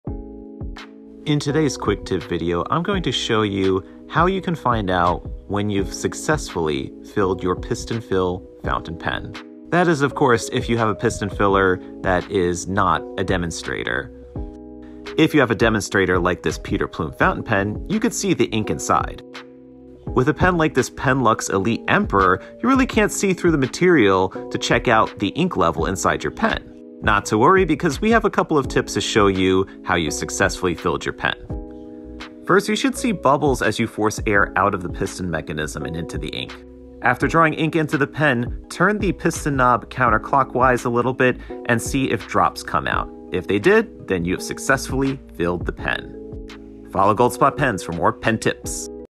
Penlux Elite Emperor Fountain Pen with Platinum Classic Citrus Black ink
While you can clearly see that you've filled your demonstrator, piston-fill fountain pen with ink, how do you know when you've successfully filled a non-demonstrator piston-fill fountain pen without an ink window. This quick tip video shows you a couple of things to look for.